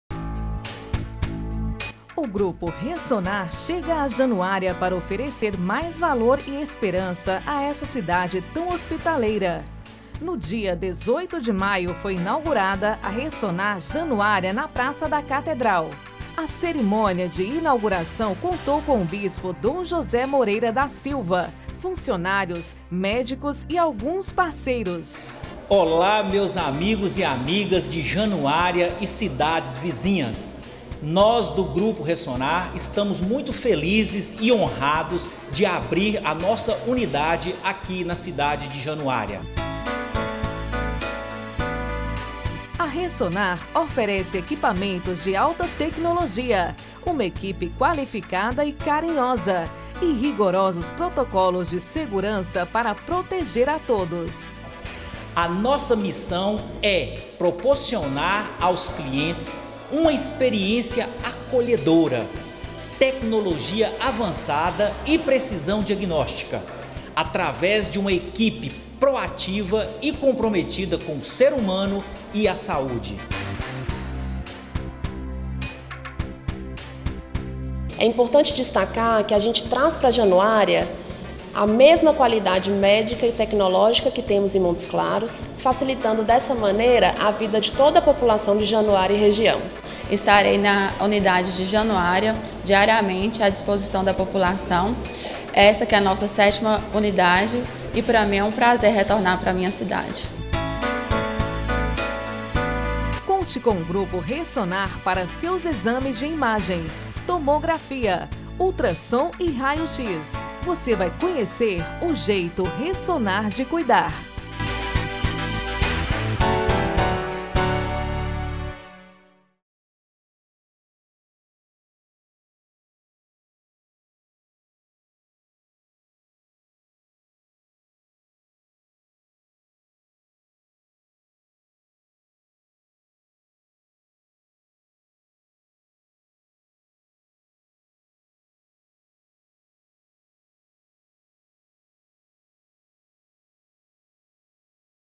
Inauguração Ressonar em Januária
É com muito prazer que inauguramos mais uma unidade Ressonar, dessa vez em Januária, estamos gratos e feliz por fazer parte dessa cidade tão acolhedora.